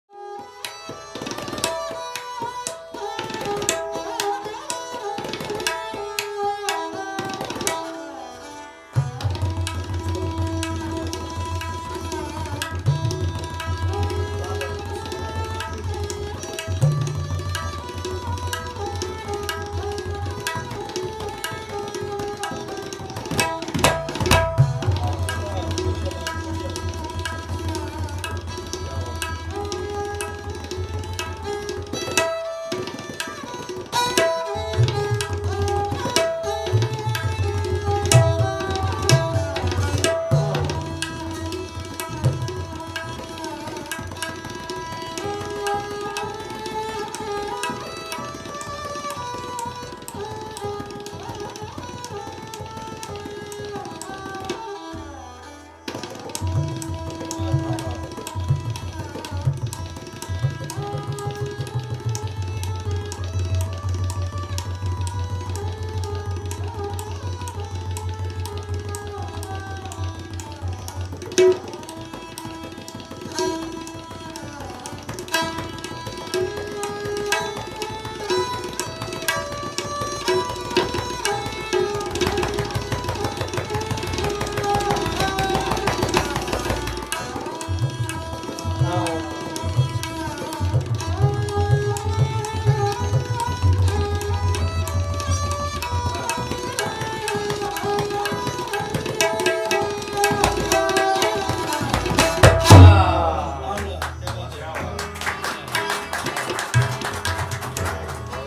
Ustad Sabir Khan & Yama Sarshar baithak sitting 2019 Part 1
A small baithak mehfil sitting at my residents with the amazing Sabir bhai. The son of the legendary Sarangi nawaz the late Ustad Sultan Khan Saeb. 🙏